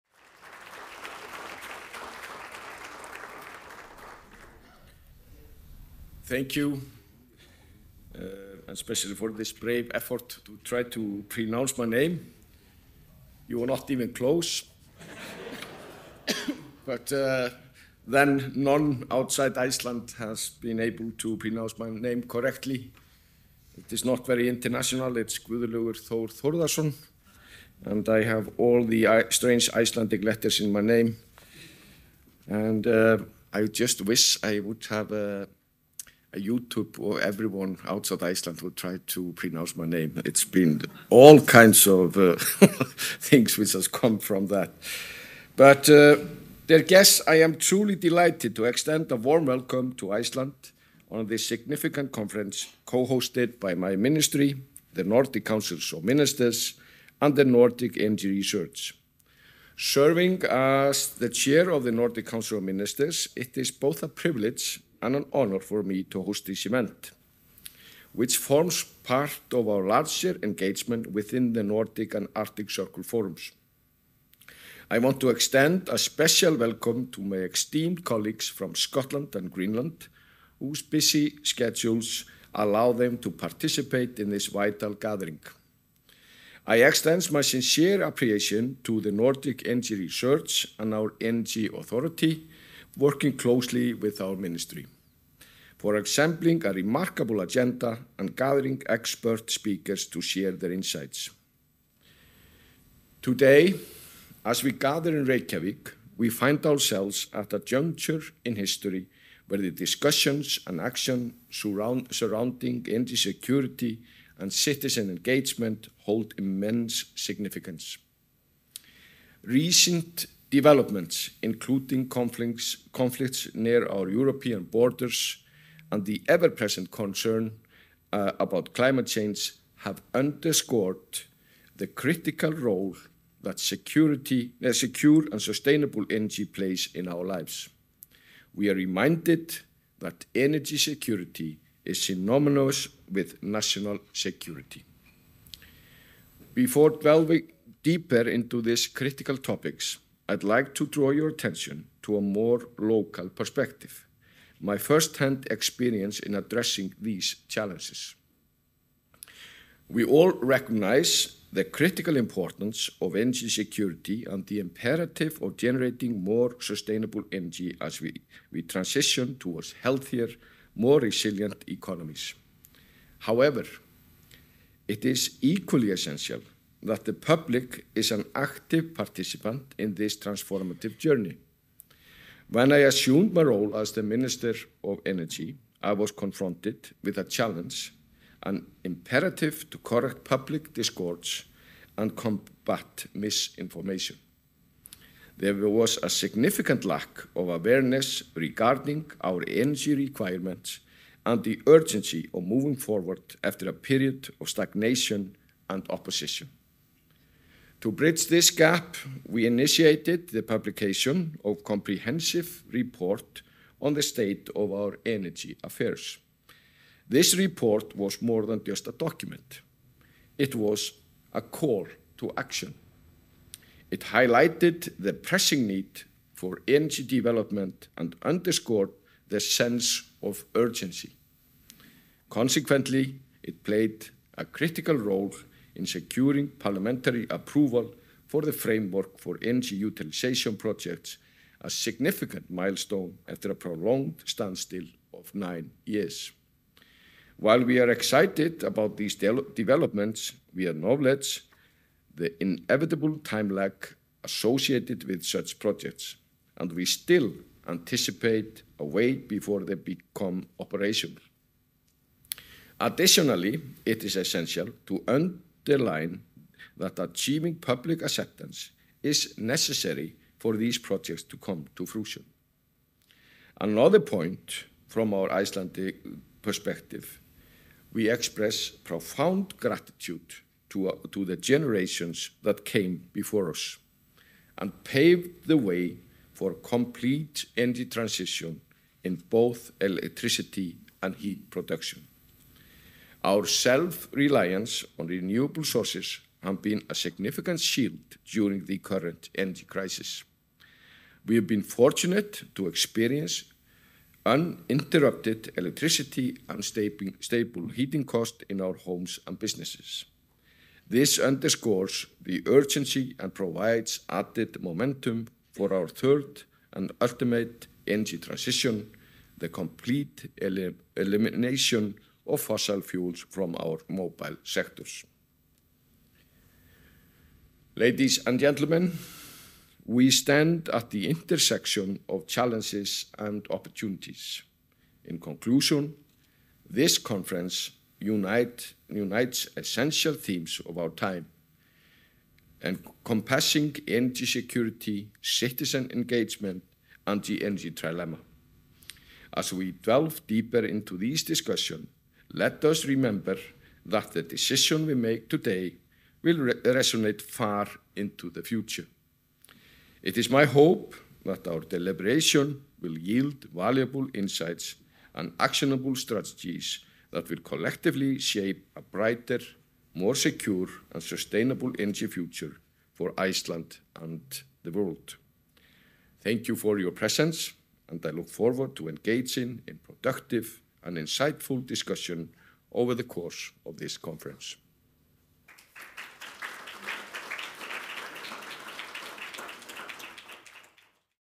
Energy Security and Citizens Conference: Welcome and opening
On 19 October 2023 in Reykjavik, Iceland, Nordic Energy Research arranged the Energy Security and Cititzens Conference, together with The Nordic Council of Ministers and The Nordic Council and the Government of Iceland, Ministry of the Enviroment, Climate and Energy.

In this video, Guðlaugur Þór Þórðarson, Minister of the Environment, Energy and Climate in Iceland, opens the conference.